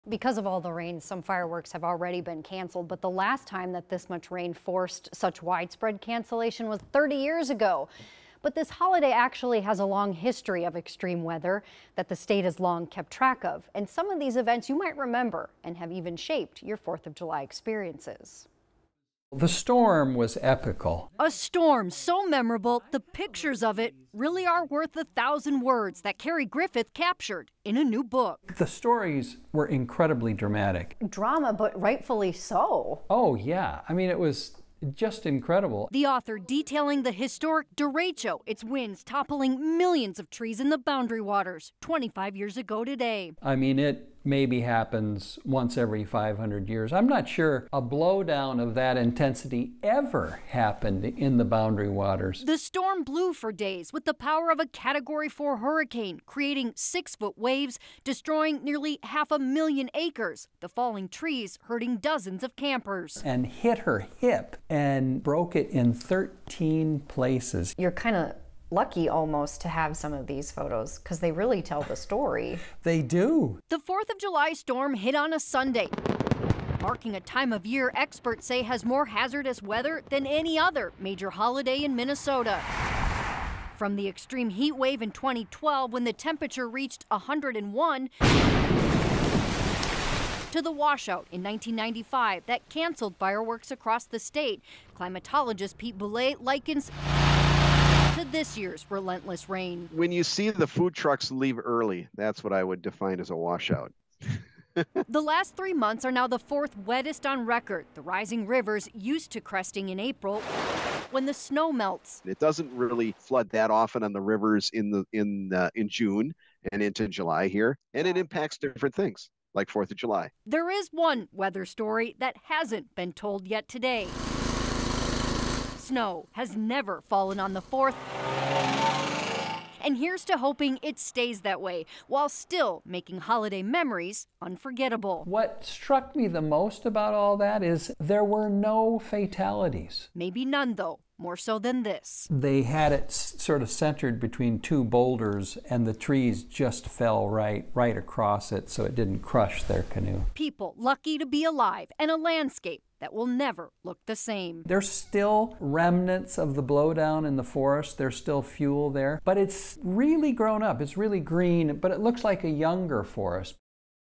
The history of hazardous weather hitting Minnesota on the 4th of July
Several record windstorms were on or near the 4th of July, including the infamous Boundary Waters Blowdown of 1999.